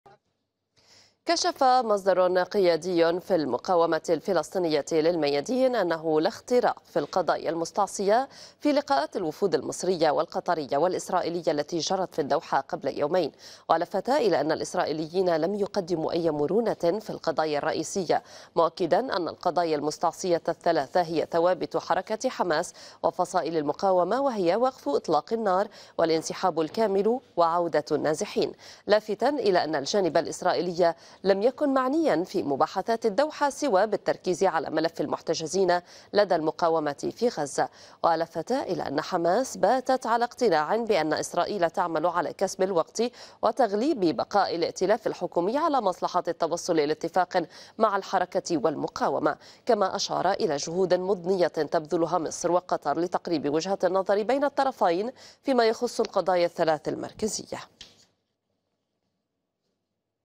مصادر المقاومة للميادين: لا حاجة إلى البقاء في المفاوضات من دون تلبية احتياجات أهل غزة
أفادت مصادر قيادية في المقاومة الفلسطينية للميادين بأنّ ثوابت المقاومة في التفاوض تشمل إغاثة الشعب الفلسطيني في قطاع غزة، على نحو بشمل الإيواء وإعادة الإعمار، وذلك ضمن المرحلة الأولى.
وبحسب المصادر، تشمل ثوابت المرحلة الأولى أيضاً، إعادة الإعمار مع التأهيل العاجل للمستشفيات والمخابز والمرافق العامة، وانسحاب قوات الاحتلال، وعودة النازحين إلى مناطق غزة والشمال، من دون قيدٍ أو شرط.
أمّا في المرحلة الثانية، فتشمل الثوابت التفاوض على الأسرى، وصولاً إلى وقف إطلاق نار شامل، مؤكدةً أنه من دون ذلك "لا يمكن للمقاومة أن تعقد اتفاقاً".
وشدّدت المصادر على أنّ "هذه المواقف الثابتة أوصلتها حماس بوضوح، باسمها وباسم المقاومة الفلسطينية"، مشيرةً إلى أنّ "قيادة حماس شدّدت على أنّ رسالتها موجهة إلى الجميع، قبل عقد أي اتفاق".